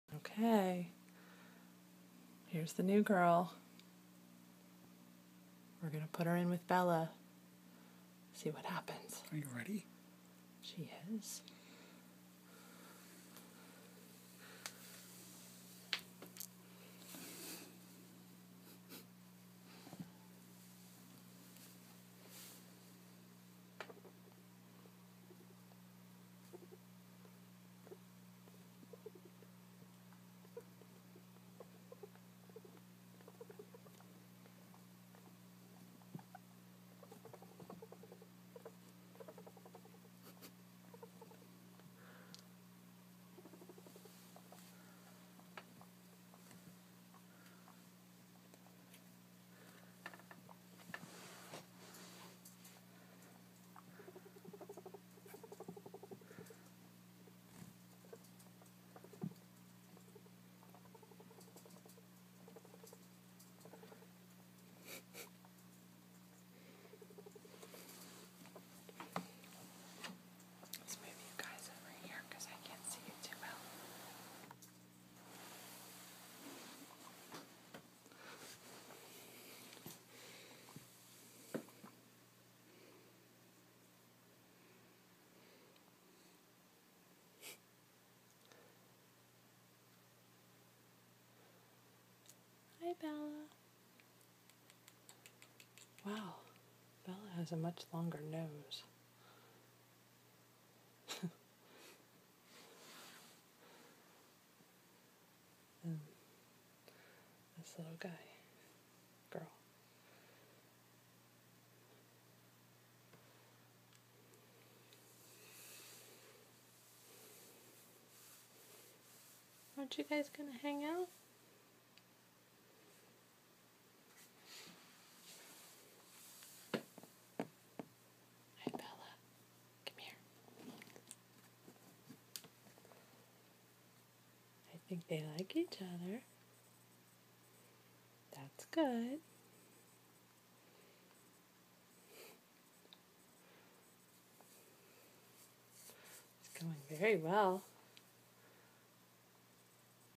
Guinea pigs meet for first time
Introducing our two 5-6 week old female guinea pigs in a neutral setting. They are now completely bonded and love each other.